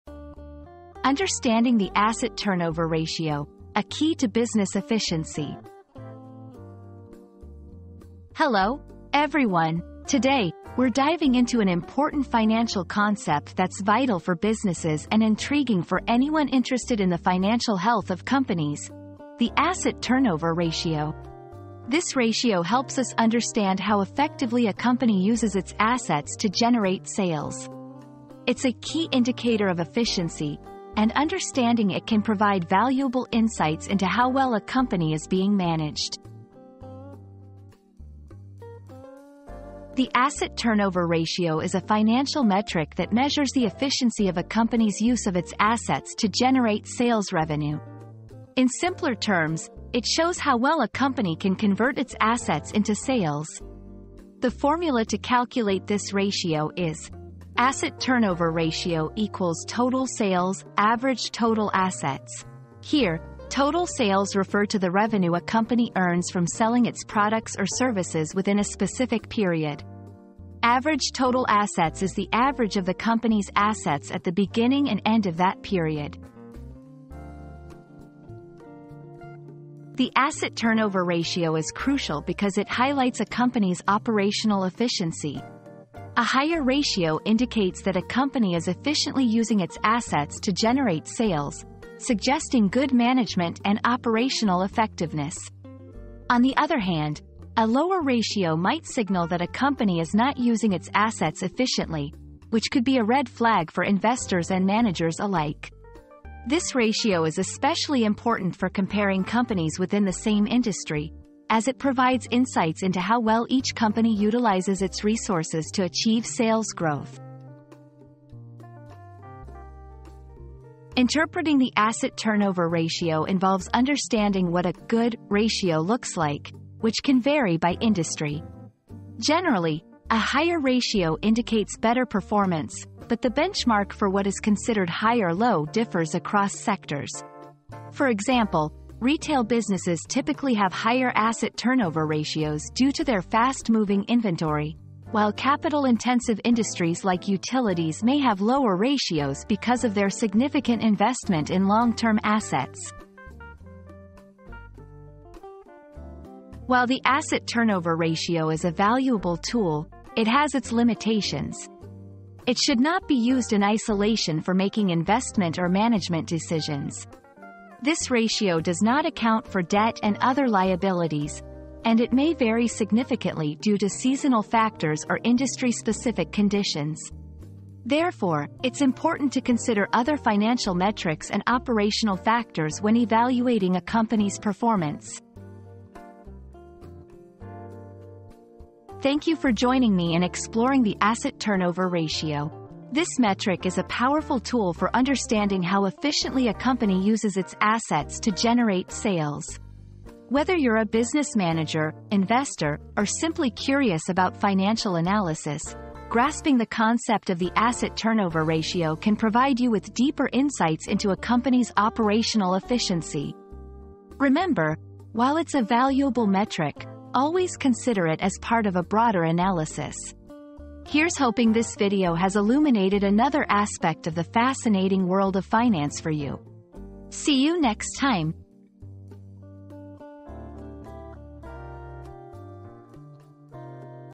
Understanding the Asset Turnover Ratio: A Key to Business Efficiency
Unlocking Business Efficiency: Mastering the Asset Turnover Ratio • Learn how to boost your business efficiency by understanding and optimizing the asset turnover ratio. Discover the key to maximizing your resources and increasing profitability.

00:00 • Introduction - Understanding the Asset Turnover Ratio: A Key to Business Efficiency
00:39 • What is the Asset Turnover Ratio?
01:19 • Why is the Asset Turnover Ratio Important?
02:00 • Interpreting the Asset Turnover Ratio
02:36 • Limitations of the Asset Turnover Ratio